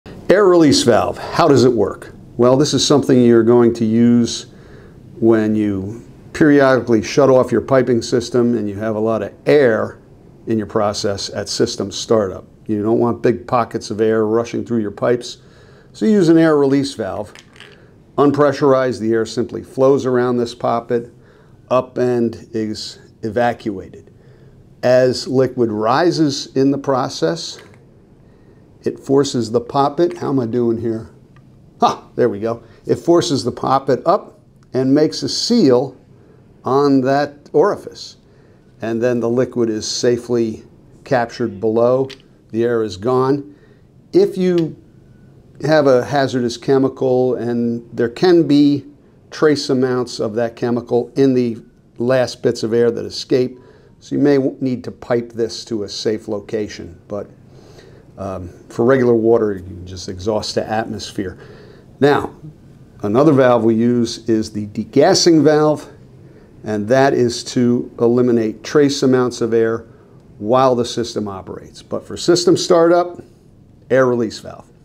How an Air Release Valve works
Designed to expel large volume of air at system start up, then close bubble tight -- the air release valve protects piping and preserves pump longevity.  Improves pump performance and system efficiency,  and reduces the danger of constricted flow and water hammer.